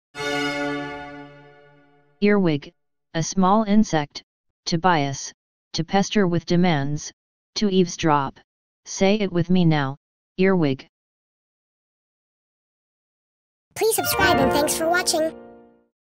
earwig - Smart & Obscure English Words Defined 🗣🔊
This video is about "earwig". Here's a video list of some of the most obscure english words with definitions that you might use as a curiosity in your conversations.